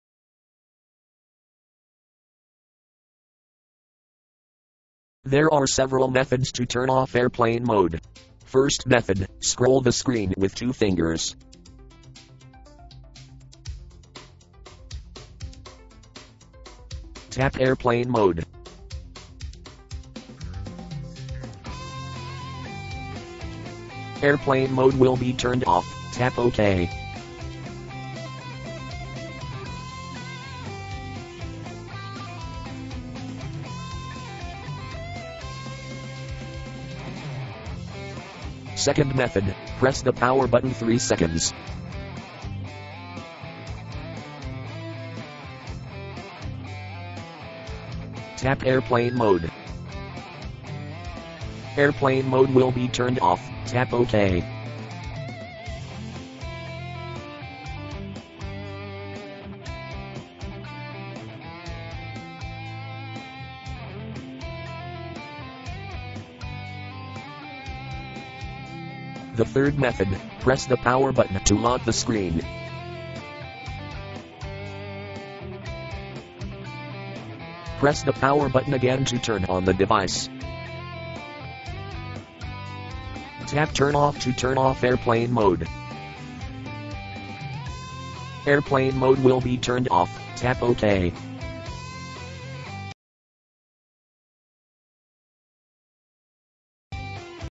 Samsung Galaxy S5 : How to turn off airplane mode (Android Phone)
This video show How to turn off airplane mode on Samsung Galaxy S5